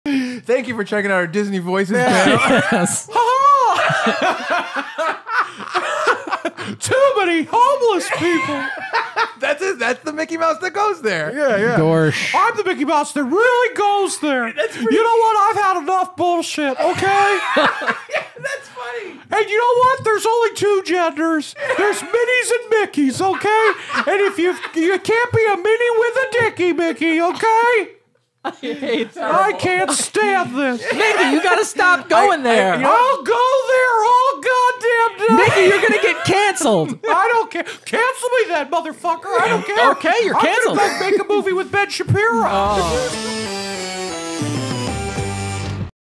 The Mickey Mouse That Goes There!
In the newest Funbearable, Ray Harrington IS ... the Mickey That Goes There!

Art courtesy of Kenny Rubenis!